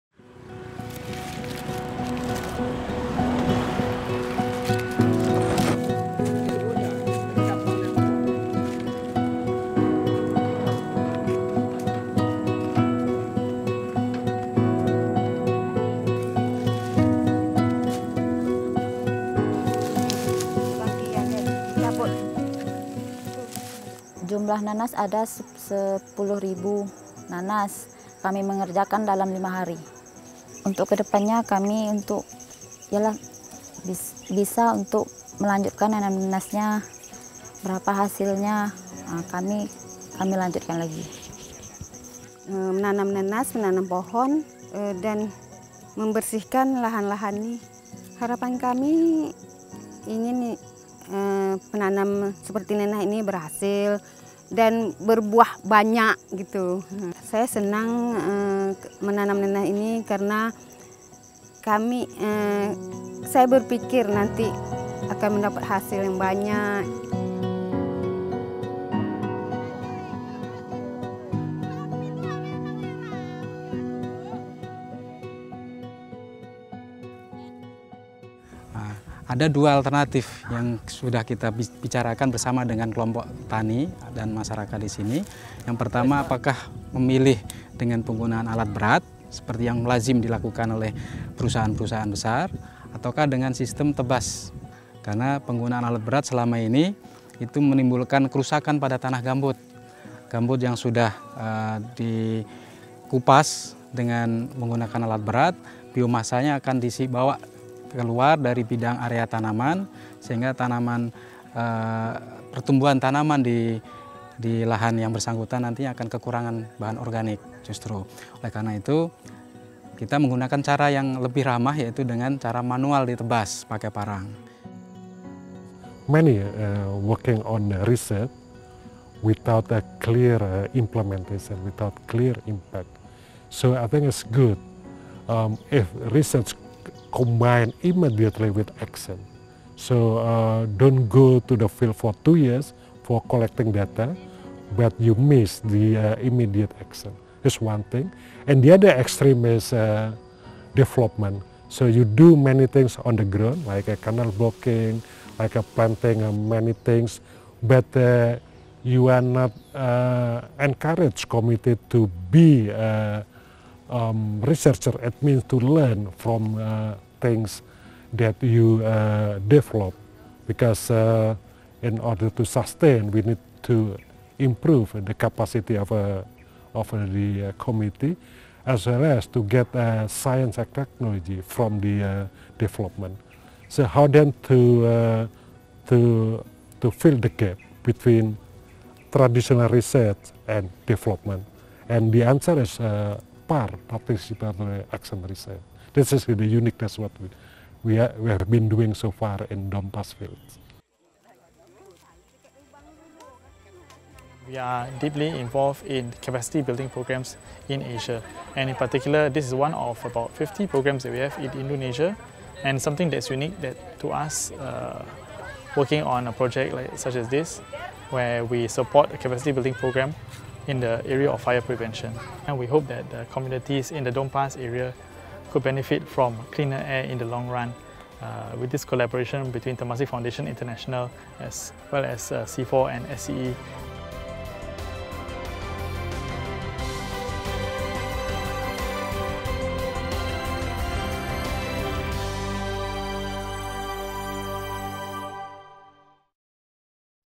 Pineapples, trees and peatlands
This work is a part of the Participatory Action Research to Community-Based Fire Prevention and Peatland Restoration, an action research project funded by Temasek Foundation International and Singapore Cooperation Enterprise. CIFOR works together with local community and partner on activities to prevent fire, restore peatland, and create sustainable livelihood. We expect to influence social transformation and changes in behavior of the local community in the long run. More information about the project is available